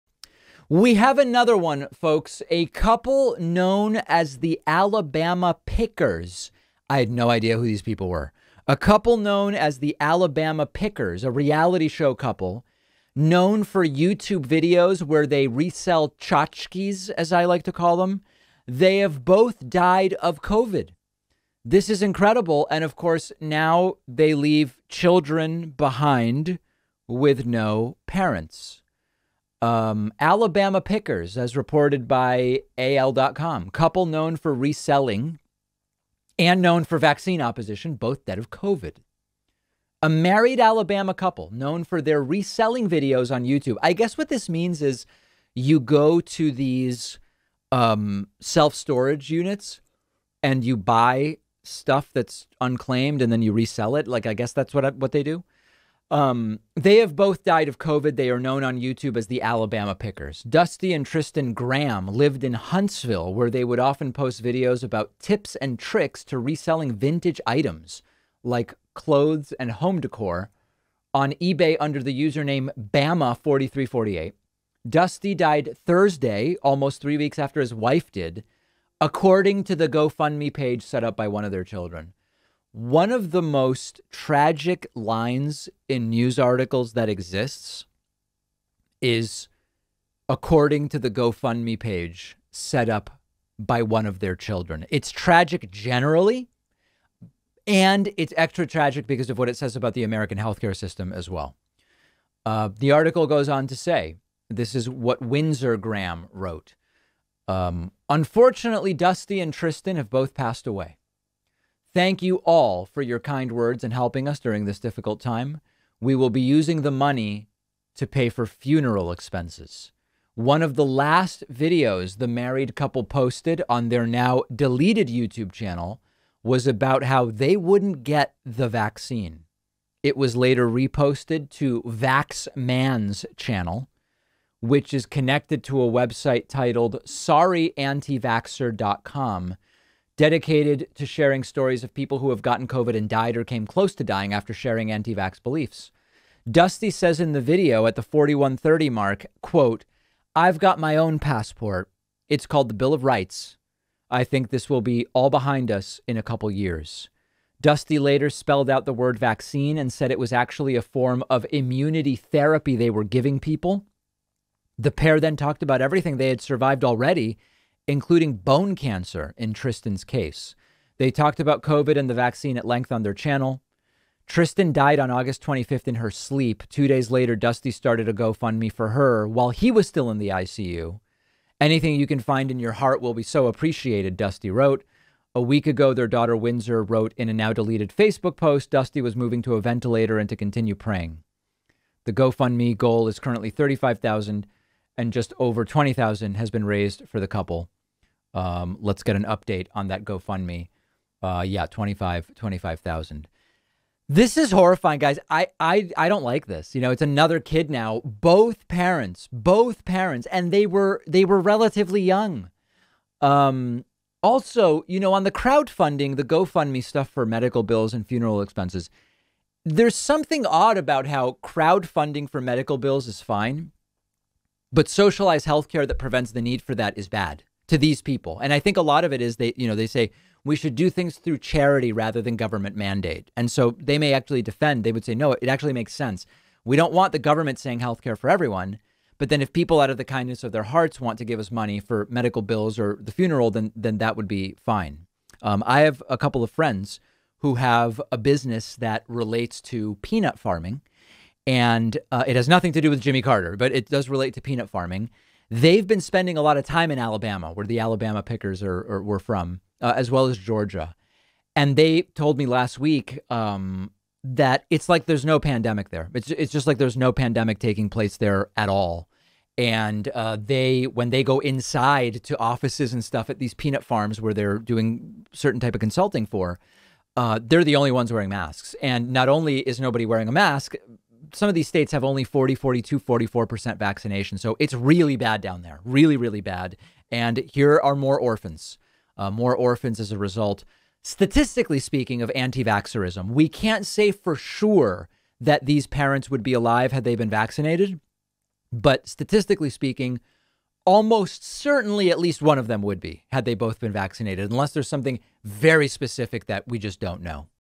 Anti-Vax Reality Show Couple BOTH Dead of COVID
--Anti-vaccine reality show couple Tristan and Dusty Graham have both died of COVID

Leave a Voicemail Line: (219)-2DAVIDP